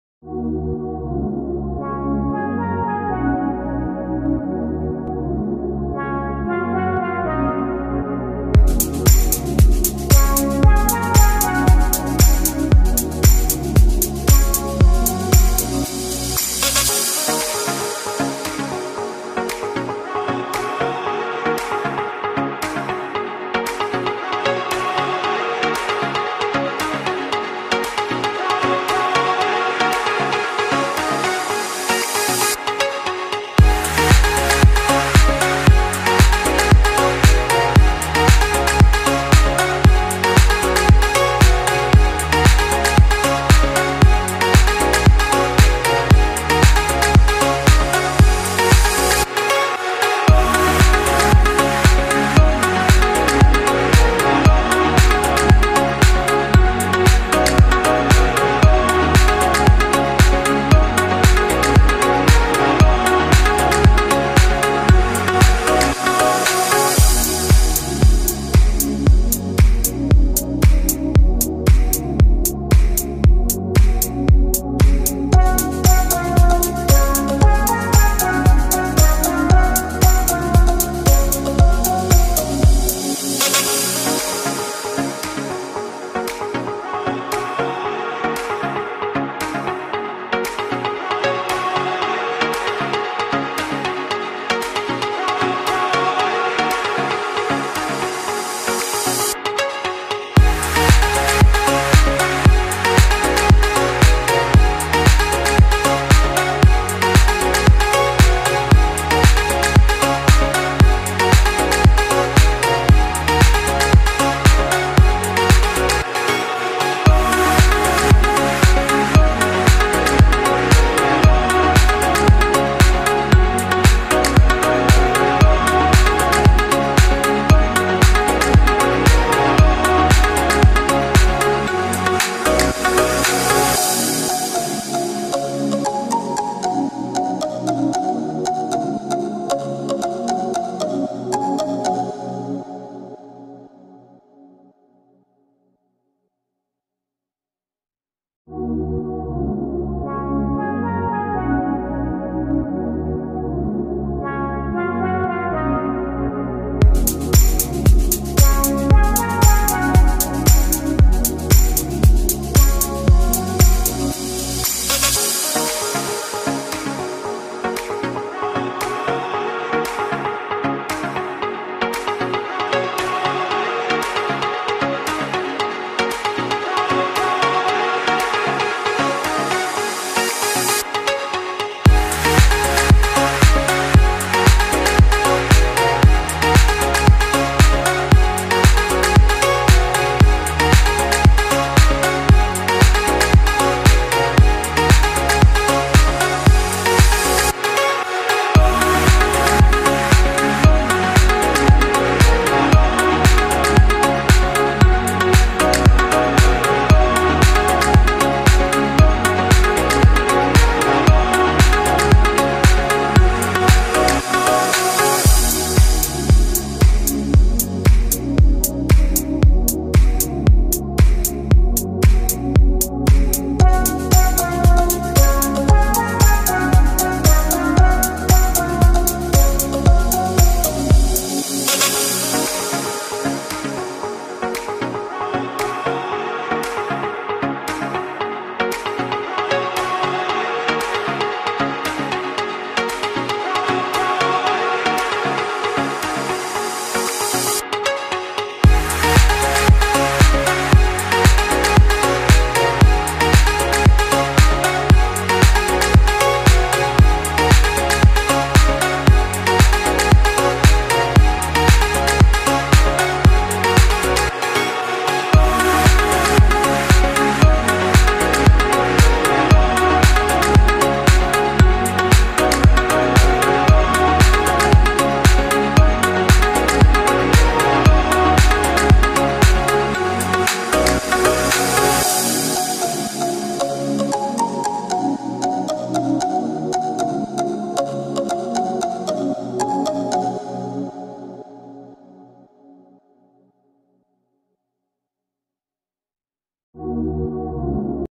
Global Forgiveness Day Poster | Drawing Painting World Forgiveness Day | Poster Idea
How to make a quick and easy poster on world forgiveness day, its very easy I have used oil pastels colours, please watch video till end & click on like.

Materials:
* drawing paper
* oil pastels( pink, rose pink, violet)
* tissue paper
* 8B / 10B pencil